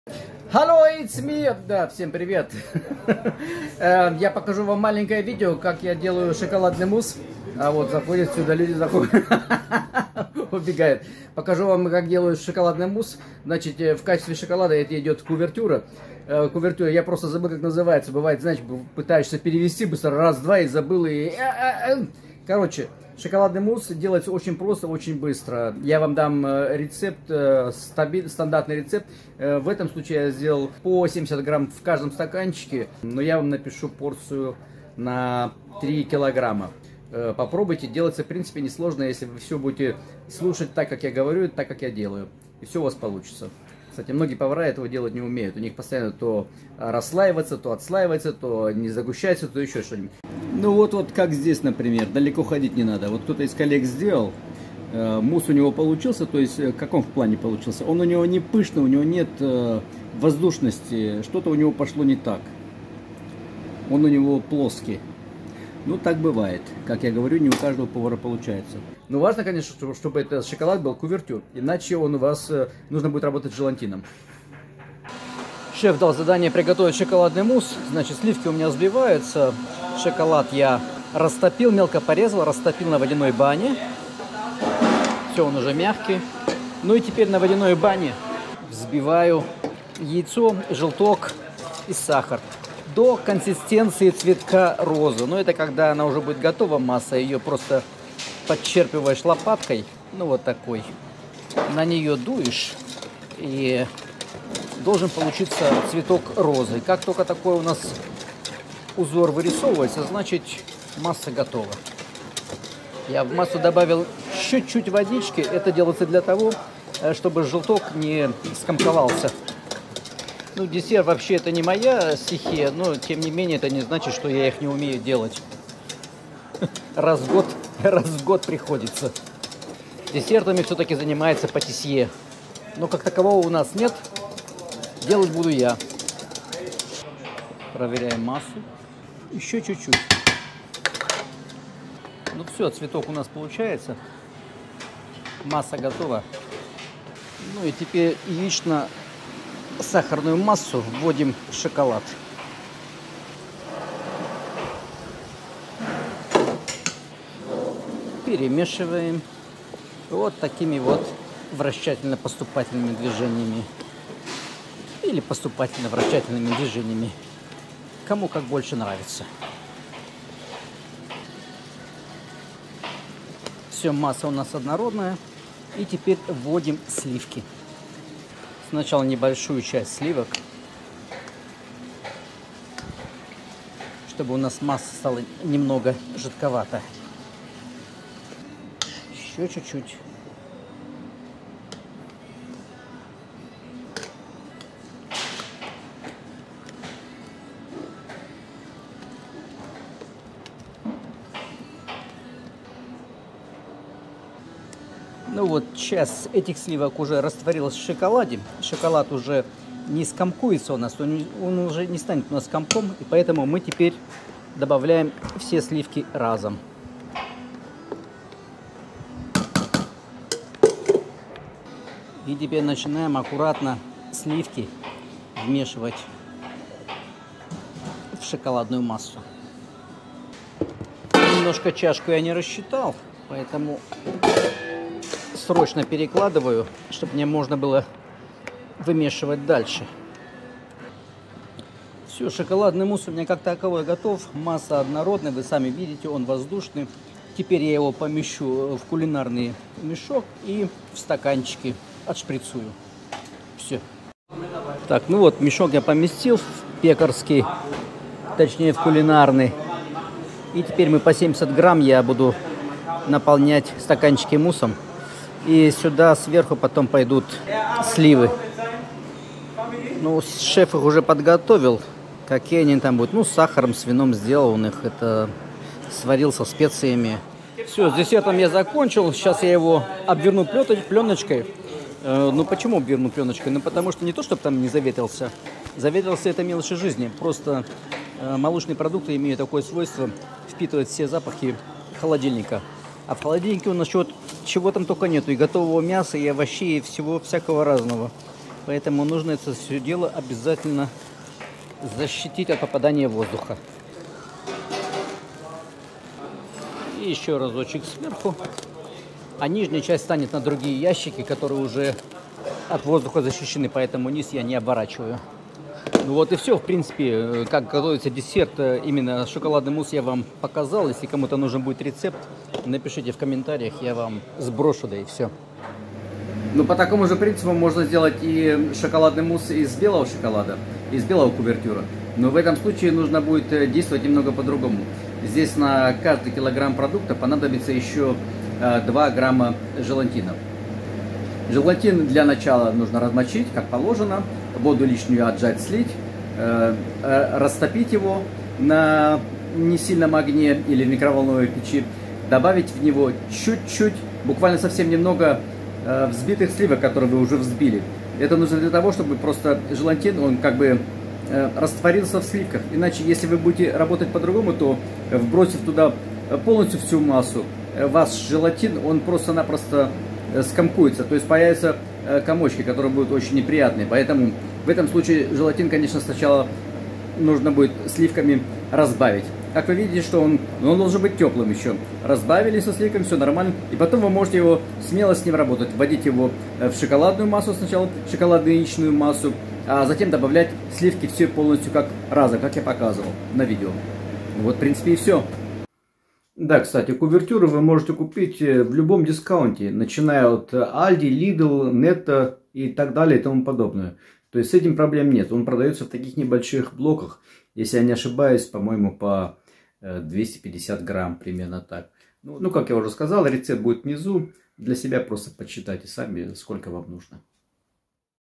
Шоколадный мусс. Десерт за 30 минут
Для рецепта нам понадобится:

125 гр желтка- 5 ​+ 50 мл холодной воды
одно яйцо (50-60 гр)
200 гр сахара
700 гр кувертюр ( шоколад )
30-35% сливки 2 литра
Всего 3,085кг

Про белый мусс я оговорился. На каждый литр(кг) массы, желатина не 2 гр ( листовой он по два грамма ) а 2 листа т.е. 4грамма на 1 литр ( 12 гр на 3,085 кг )